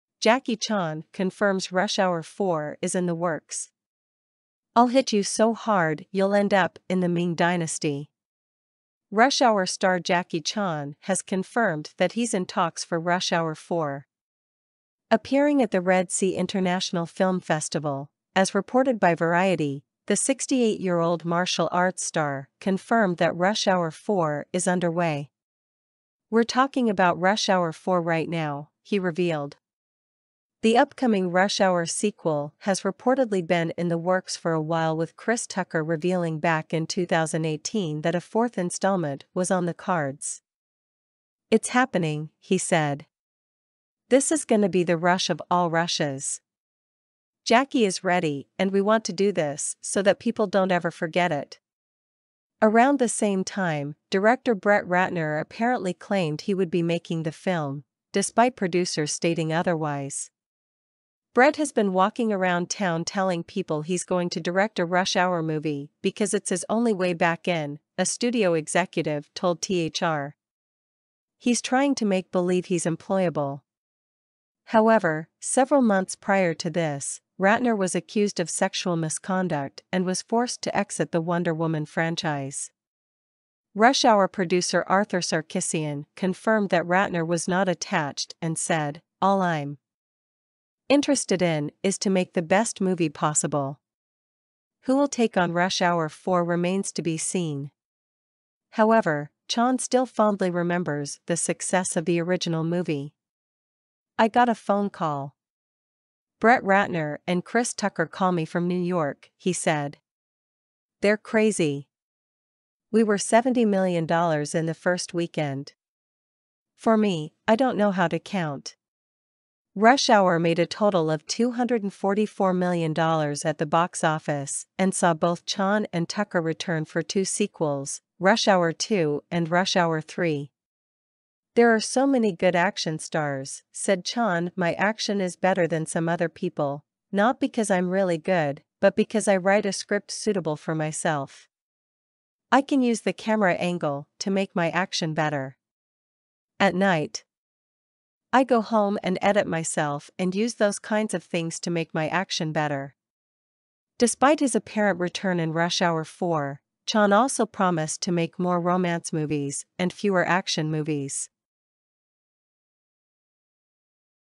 Jackie Chan Confirms Rush Hour 4 Is in the Works
Rush Hour star Jackie Chan has confirmed that he's in talks for Rush Hour 4. Appearing at the Red Sea International Film Festival, ...